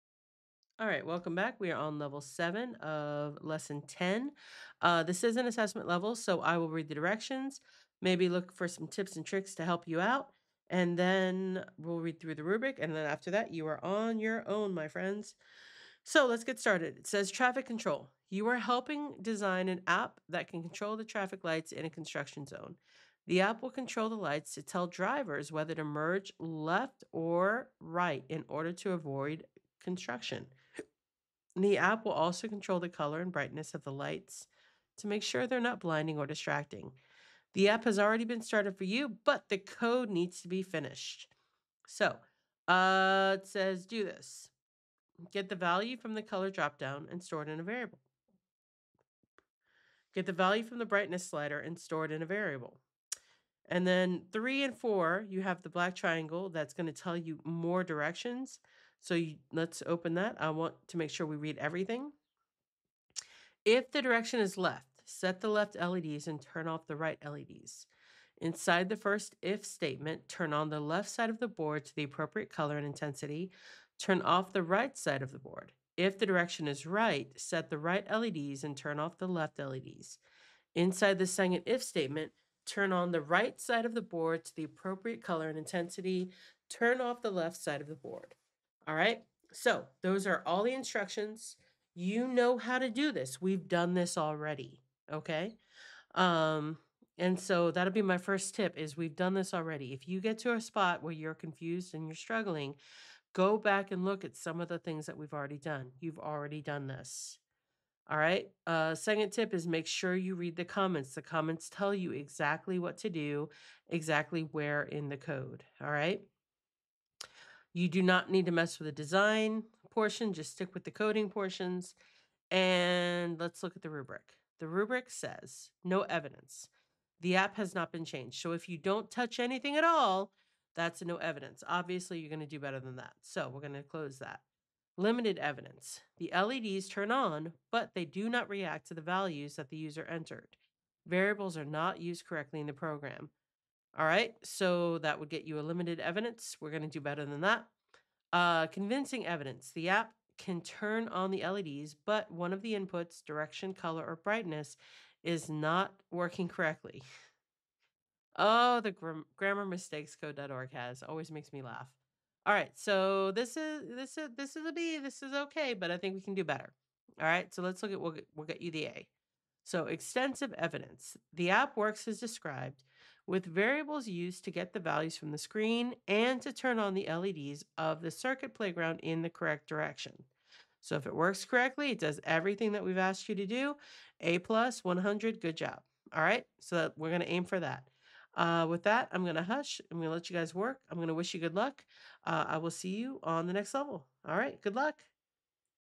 Lesson 10: Combining Inputs and Outputs - Level 7 - Creating Apps with Devices - Circuit Playground
Hello! My name is Ms. Nunes and I am a Computer Science teacher working with Middle School students. This year I am working with Code.org's CS Discoveries curriculum and as it has been updated with new activities, I'm making tutorial videos for students who would like that extra resource.

This video covers Code.org - Creating Apps with Devices - Circuit Playground: Lesson 10: Combining Inputs and Outputs - Level 7

This is an assessment level, so while I'll read through it with you and go over the rubric (and offer a hint or two), I am not going to go through each step of solving this. It wouldn't be a very good assessment if I did that. :)